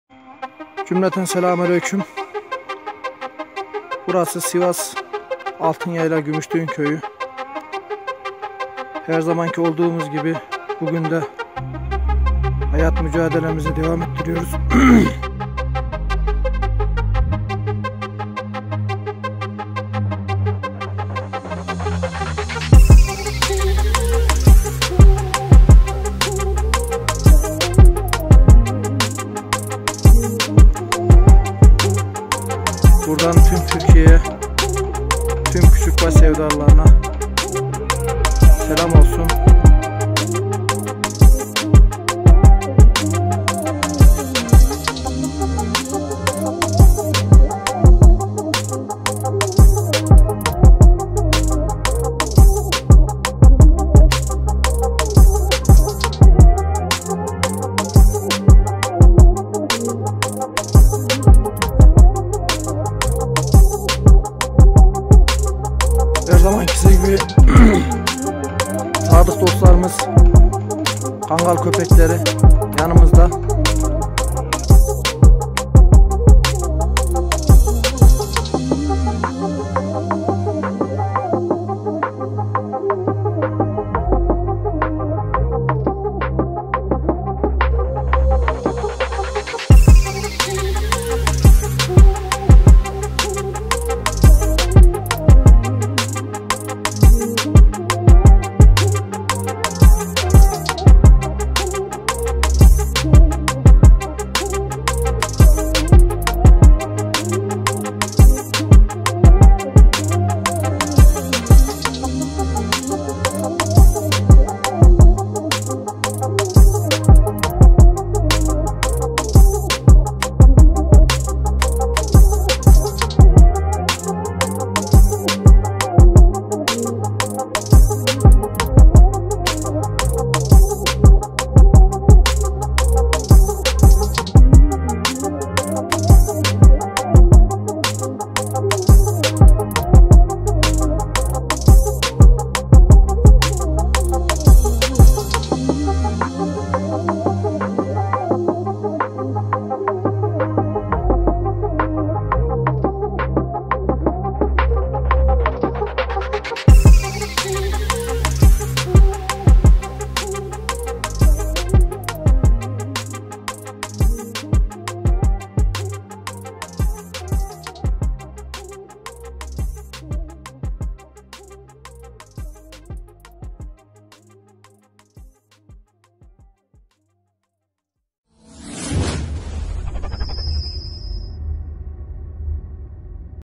Kangal Köpekleri 2019 Karda Koyun Sürüsü Çoban Hayatı Sivas | Serhatlı
Bir çobanın nasıl ekmek kazandığını gösteren koyun sürüsü, kangal köpekleri ve çoban hayatı ile ilgi bu videoyu sizlerle paylaşma gereği duydum umarım beğenirsiniz. Arkadaşlar Ev hayvanları videoları, yabani hayvanlar videosu kangal köpek videoları ve buna benzer videolar istiyorsanız. Umarım keyif alırsınız iyi seyirler.